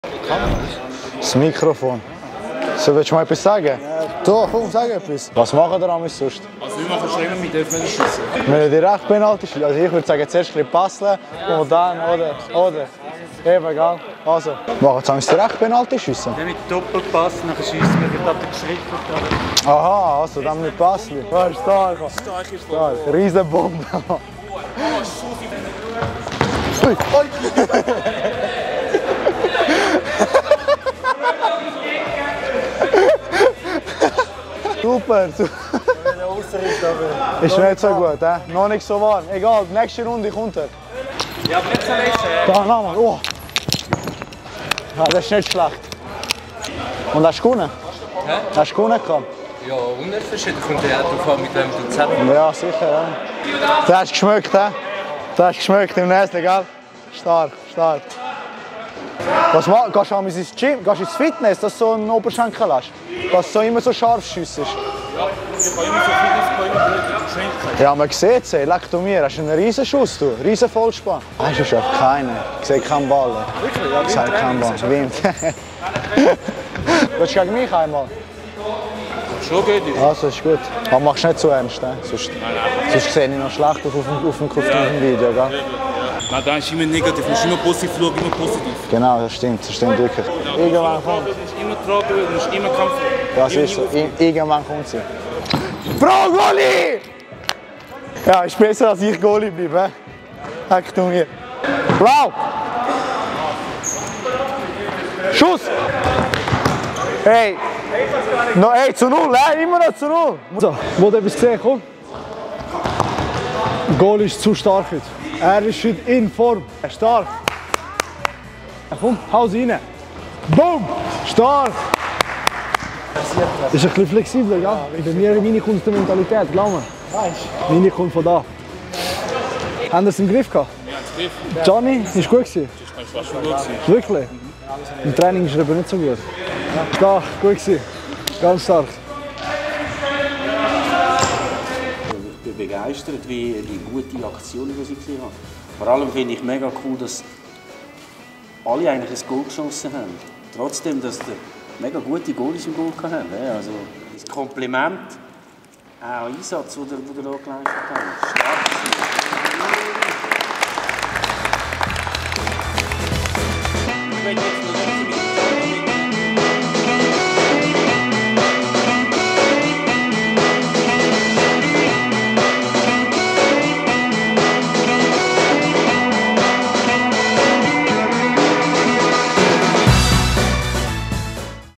De Nitti und Kamberi - Mic'd Up! 🔊 I Training FCZ Brunau
"Häsch ja en riese Schuss, wow!" ☄️

Wir haben Lindrit Kamberi und Gianni De Nitti bei ihrem Trainingsbesuch beim FCZ Brunau mit einem Mikrofon verkabelt. 🎤